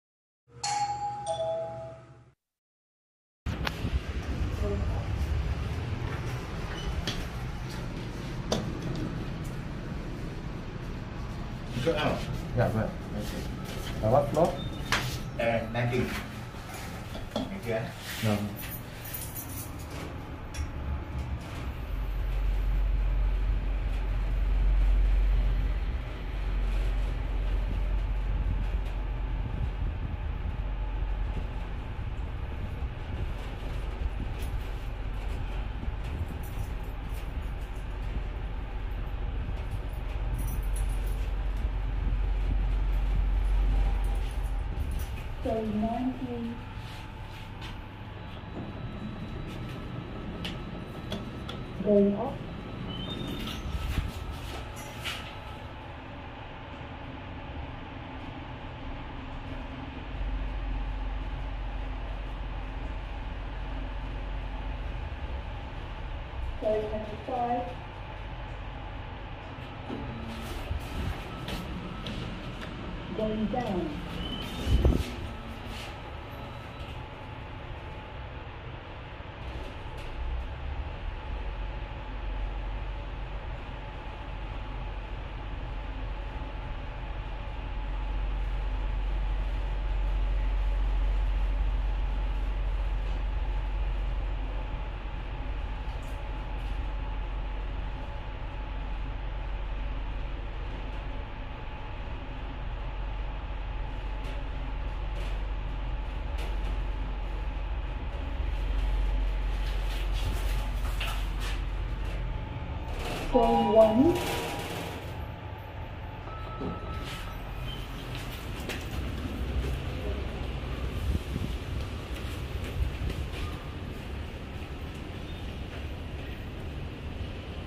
Block 700C Ang Mo Kio Residential HDB - Mitsubishi Elevators
Details :

Brand : Mitsubishi
Capacity : 13 Persons
Load : 900 kg
Floors served : 1 to 25
History : Original